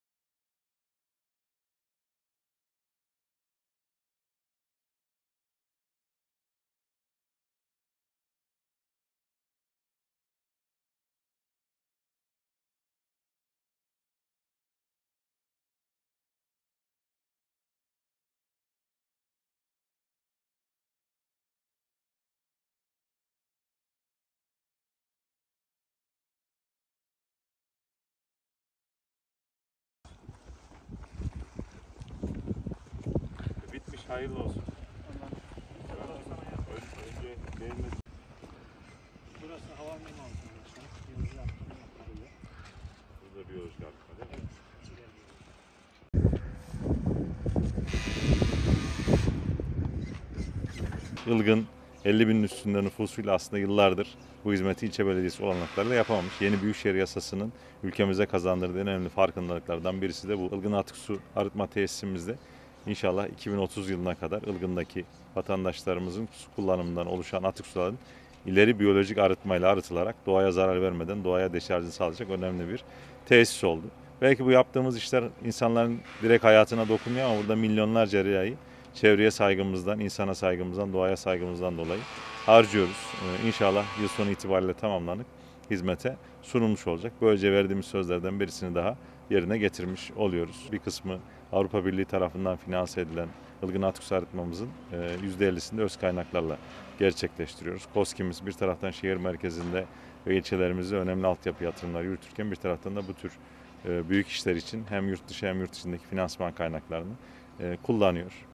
BAŞKAN ALTAY: “ILGIN’DA ÇEVRE DOSTU BÜYÜK BİR YATIRIM YÜRÜTÜYORUZ”
Konya Büyükşehir Belediyesi, Ilgın’a ileri biyolojik atıksu arıtma tesisi kazandırıyor. Yapımı son aşamaya gelen tesiste çalışmaları yerinde inceleyen Konya Büyükşehir Belediye Başkanı Uğur İbrahim Altay, Ilgın’da çevre dostu  büyük bir yatırım yürüttüklerini belirterek, “Ilgın Atıksu Arıtma Tesisi’miz atık suların ileri biyolojik arıtmayla arıtılarak doğaya zarar vermeden, doğaya deşarjını sağlayacak önemli bir tesis oldu. Böylece vermiş olduğumuz bir sözü daha yerine getirdik” dedi.